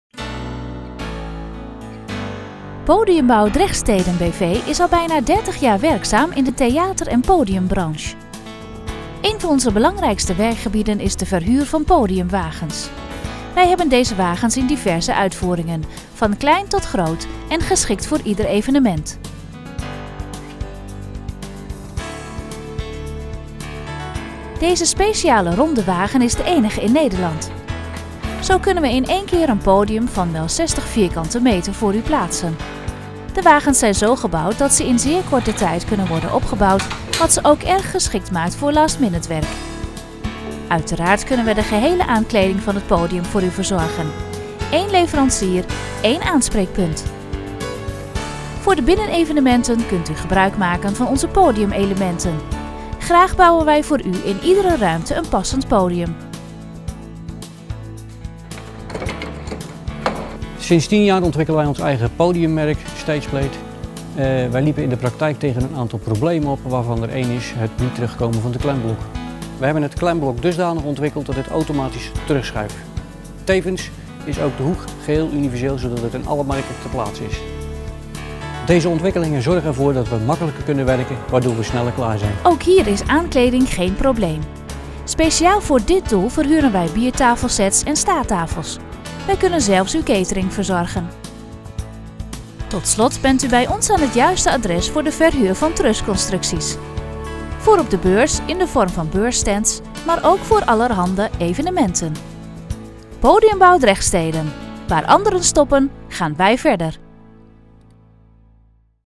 Bedrijfsfilm Podiumbouw Drechtsteden
Podiumbouw Drechtsteden b.v. is al bijna 30 jaar werkzaam in de theater en podium branche. Een van onze belangrijkste werkgebieden is de verhuur van podiumwagens. Wij hebben deze wagens in diverse uitvoeringen. Van klein tot groot en geschikt voor ieder evenement. Deze speciale ronde wagen is de engie in nederland. Zo kunnen we in een keer een podium van wel 60 m2 meter voor u plaatsen. De wagens zijn zo gebouwd dat ze in een zeer korte tijd kunnen worden opgebouwd, wat ze ook erg geschikt maken voor 'last minute' werk. Uiteraard kunnen wij de gehele aankleding van het podium voor u verzorgen. 1 leverancier, 1 aanspreekpunt

Voor de binnenevenementen kunt u gebruik maken van onze podiumelementen. Graag bouwen wij voor u in iedere ruimte een passend podium.

"stukje eigenaar"

Ook hier is aankleding geen probleem. Speciaal voor dit doel verhuren wij biertafel sets en statafels Wij kunnen zelfs uw catering verzorgen.

Tot slot bent u bij ons aan het juiste adres voor de verhuur van trussconstructies. Voor op de beurs, in de vorm van beurs stands. Maar ook voor allerhande evenementen.

Alle materialen die verhuurd worden zijn uiteraard ook in de verkoop verkrijgbaar.

Podiumbouw Drechtsteden, waar anderen stoppen gaan wij verder!

Dhr. G. Knoop
Grevelingenweg 5
3313 LB Dordrecht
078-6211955

Ook een bedrijfsfilm?